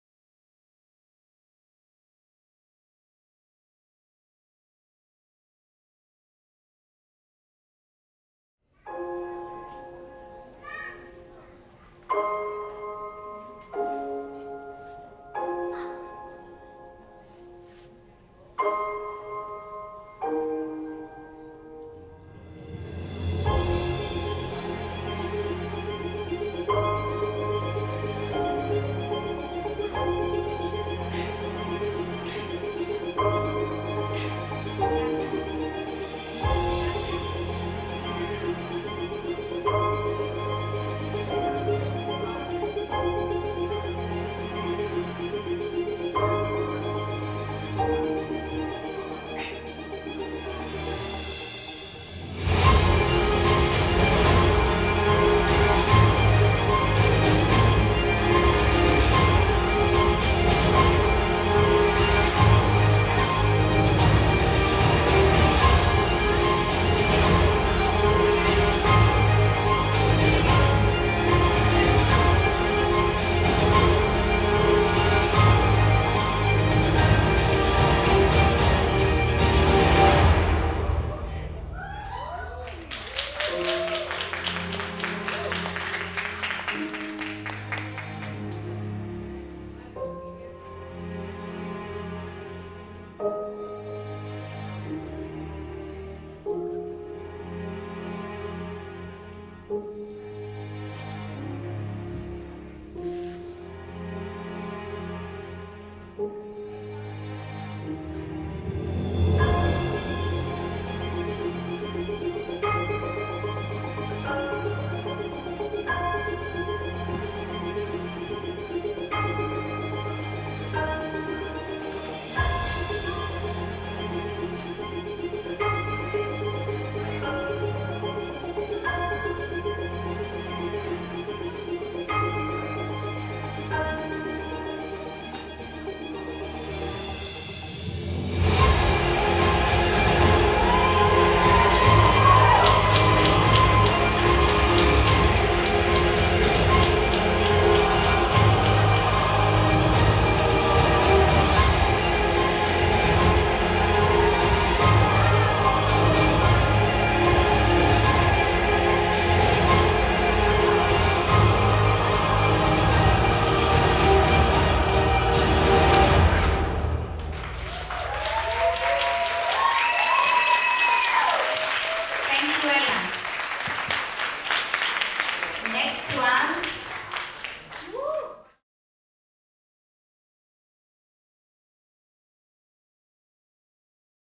Cairo by Night 2023 Ella Voi Dance
Cairo by Night Festival 2023
Organized by Stella Zahir
My Voi Performance and  Place 2 ! in the Competition
Choreography : Delanna Dance
Video: Vladimir Kluchenkov